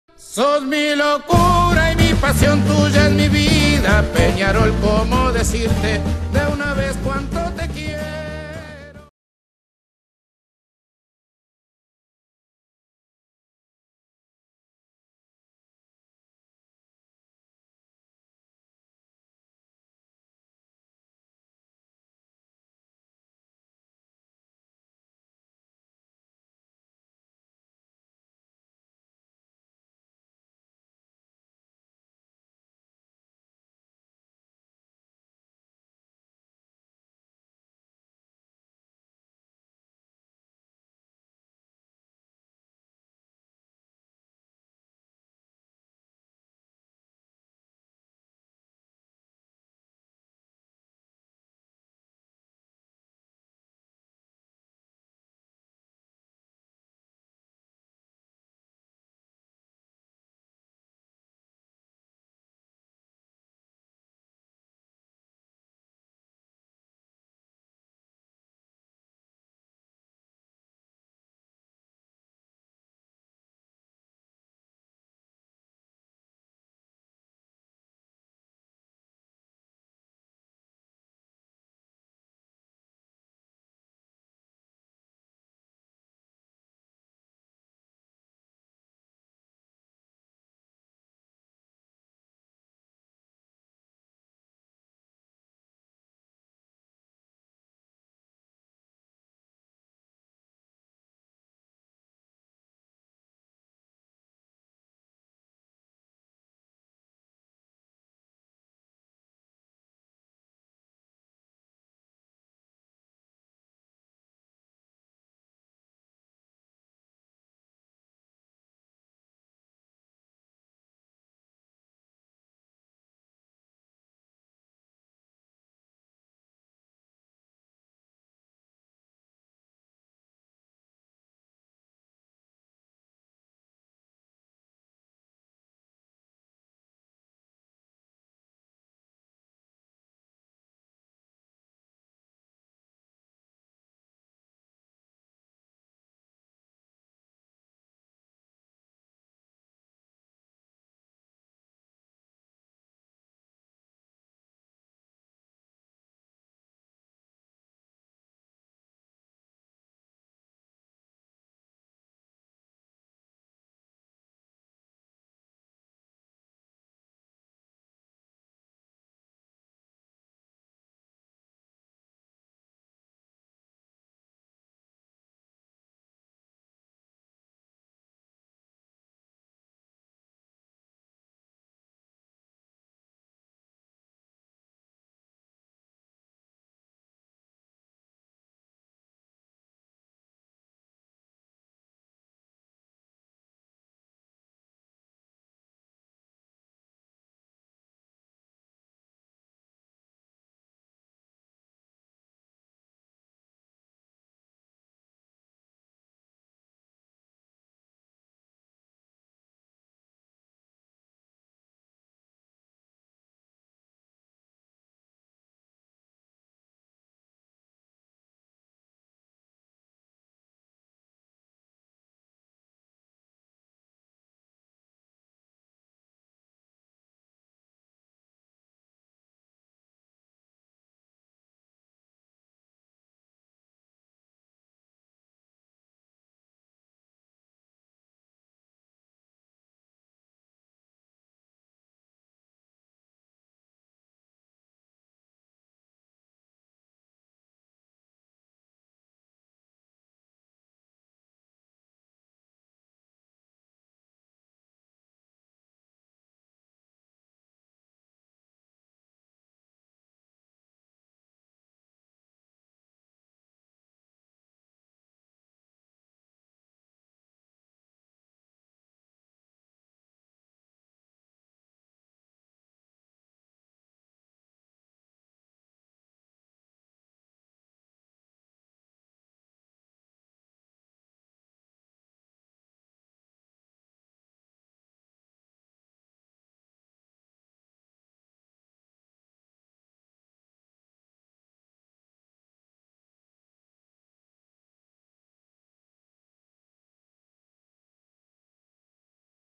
Nueva Incorporación Al Carbonero (Luis Maldonado) // Manya Serás Eterno
Hola Manyaaas!! Acá le dejo un resumen de algunas jugadas de la nueva incorporación al club (Luis Maldonado). Espero que les guste dejenme su apoyo con un like y una suscripción!! Muchas Gracias!!!